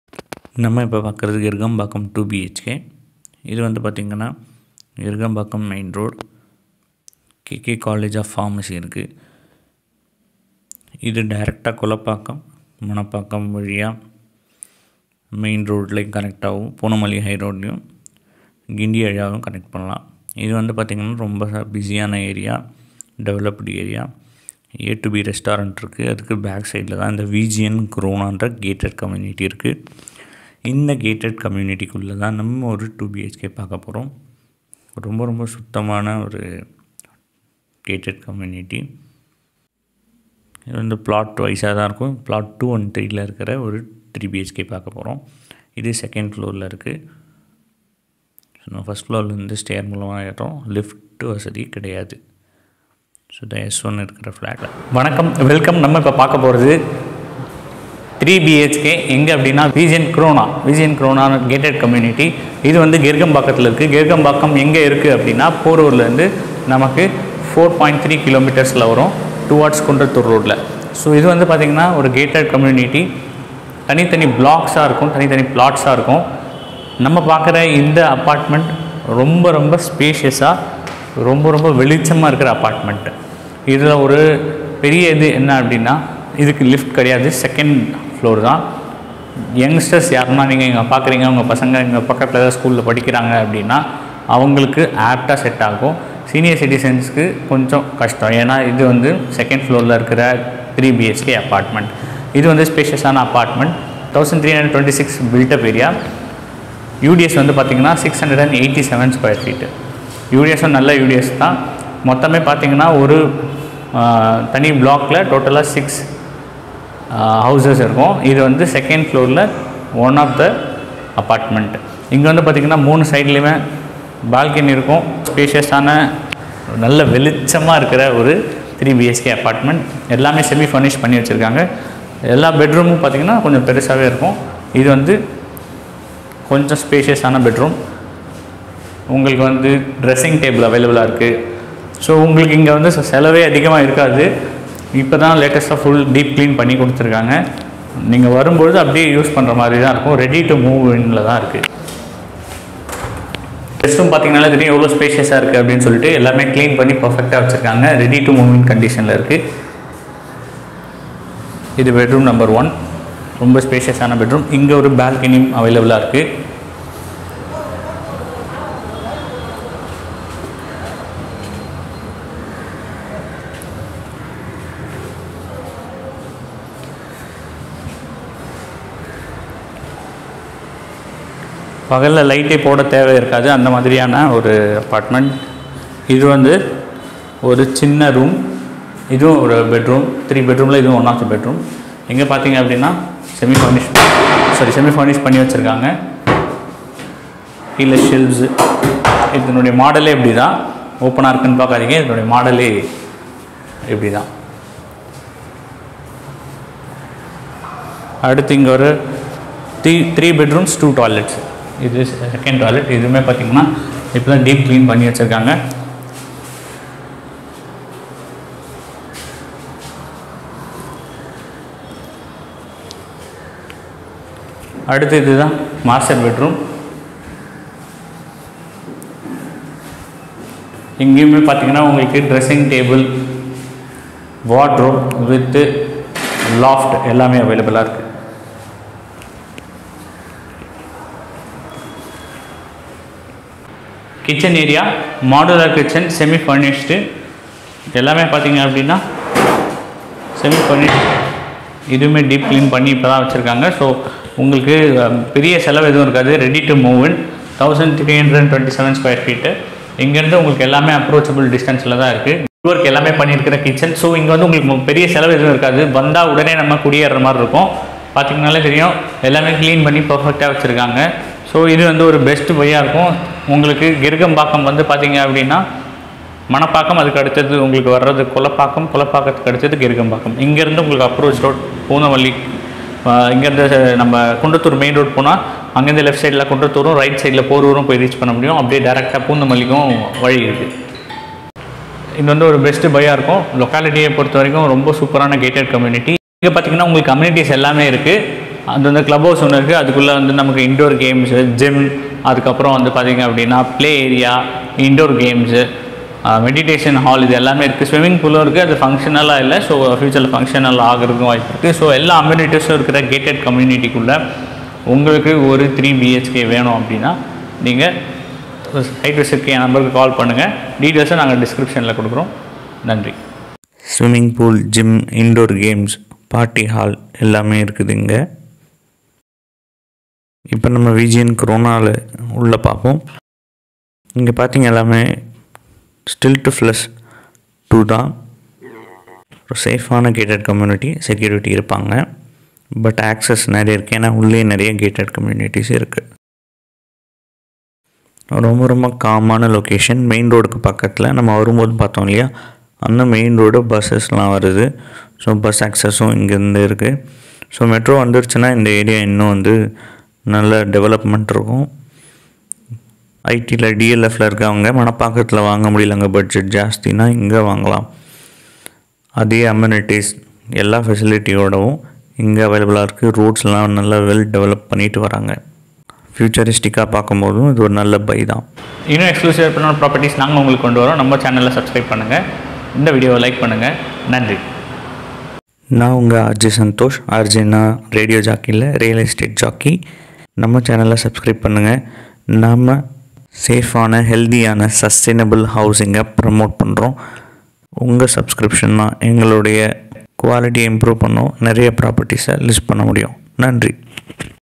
Flats for sale in Chennai Gerugambakkam , Near Porur Gated Community with all amenities VGN KRONA
Gated community apartments in chennai Gerugambakkam, Porur with all amenities

3 BHK For Sale @ Gerugambakkam, Near Porur

Second floor
9 years
65 Lakhs
No Lift
1 Covered Car Park
Swimming Pool
Gym
Party Hall
Garden Area

Chapters:
00:00 - Locality Tour
00:42 - Flat Tour
05:23 - VGN Krona Amenities and Gated Community tour

ReJ Santhosh Chennai (Real  Estate Jockey Chennai)
Realtor @Hanureddyrealty
Hanu Reddy Realty
International Real Estate Brokers